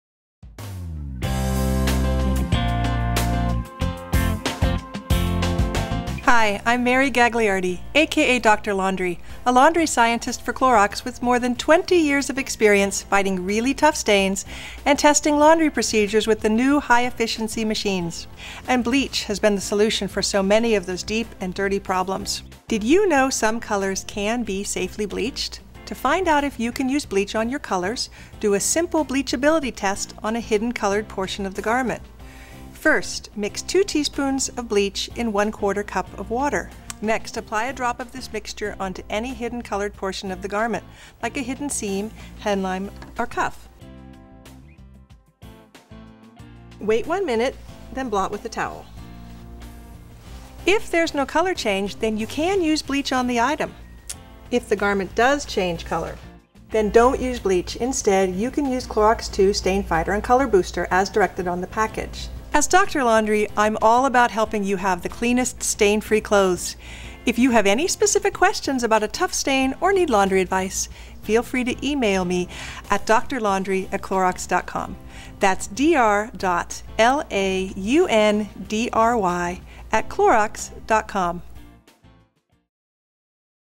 Clorox Presents Dr. Laundry Bleach for Beginners: Bleachability Test
Dr. Laundry shows you how to perform a bleachability test.